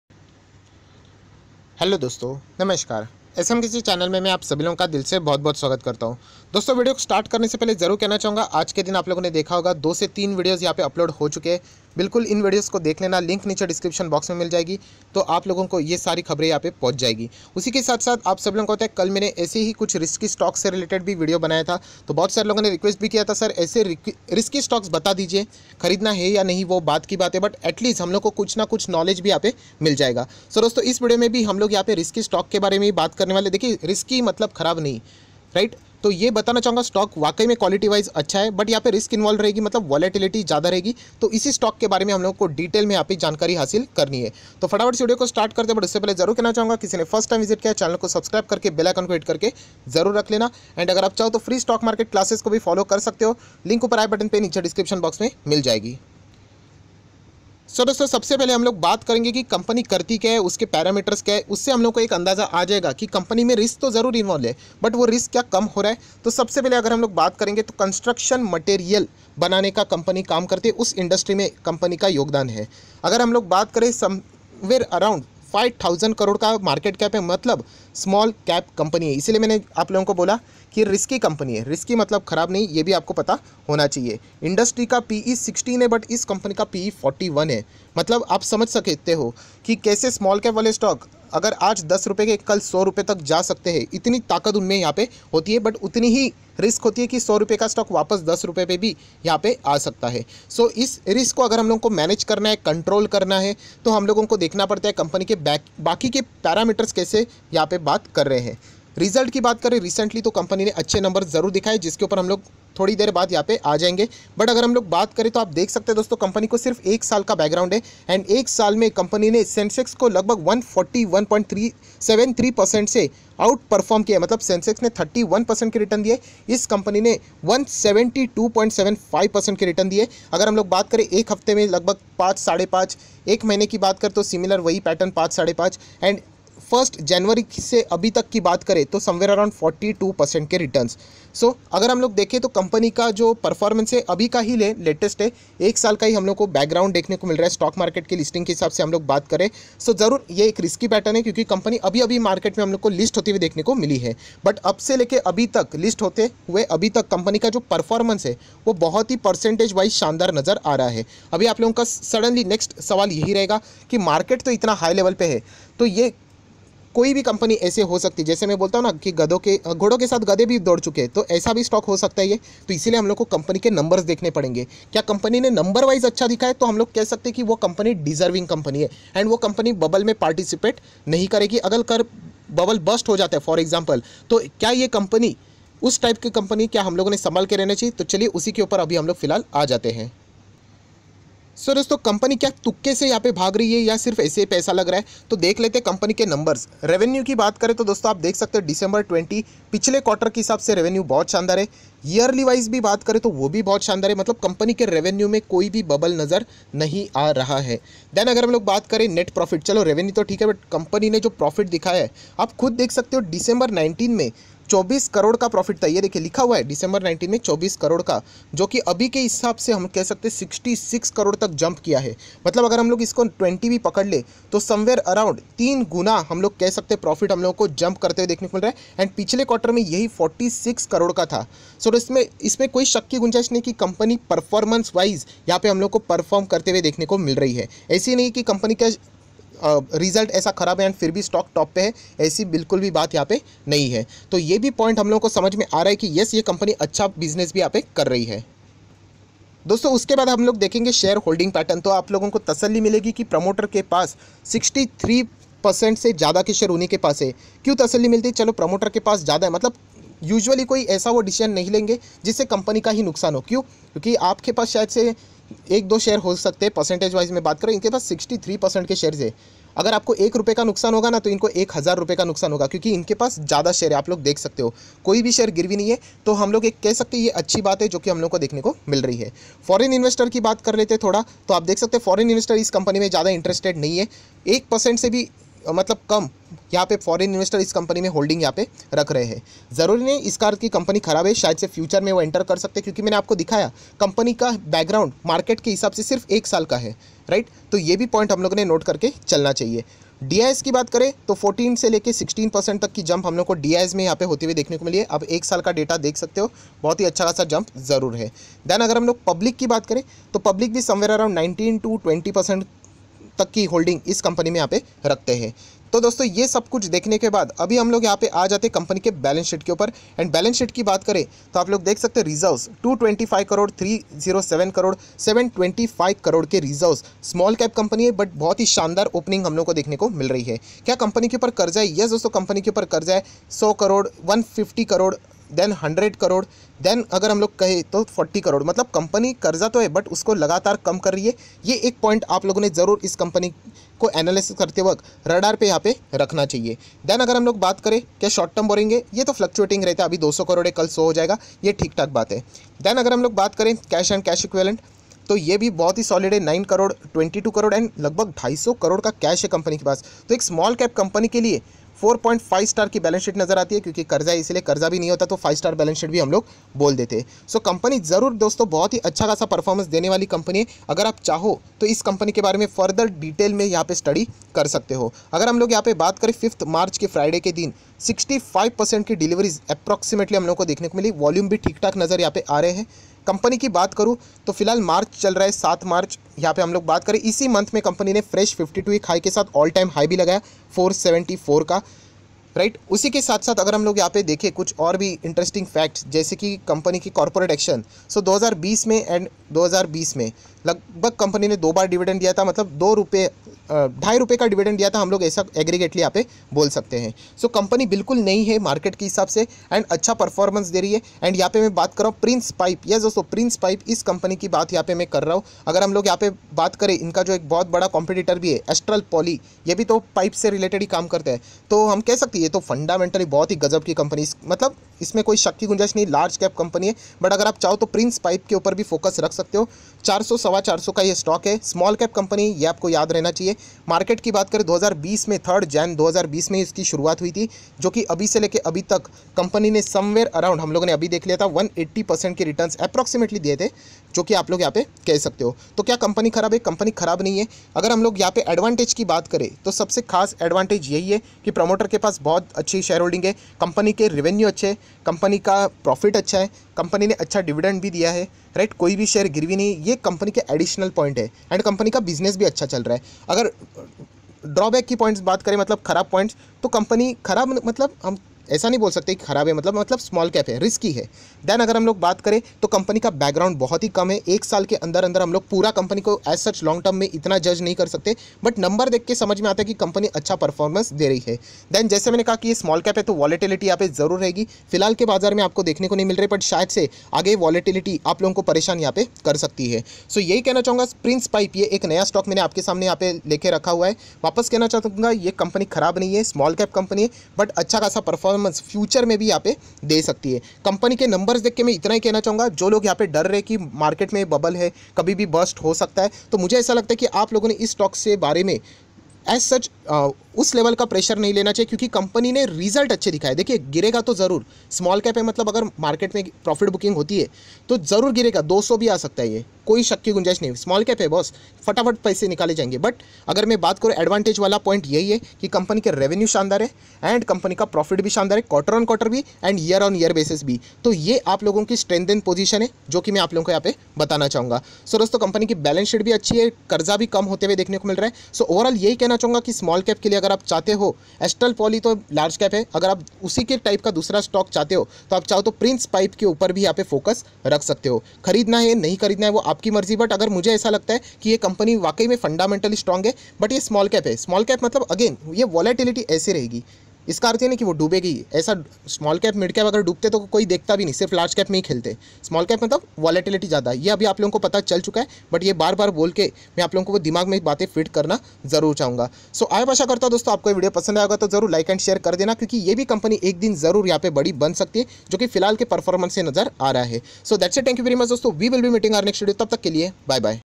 आज Stock Rs 500 के नीचे मिल रहा है भविष्य में नहीं मिलेगा (Stock below 500 rs) Prince Pipes 426 SMKC
Free Stock Market Classes :   @STOCKMARKETCLASSES

Ty :)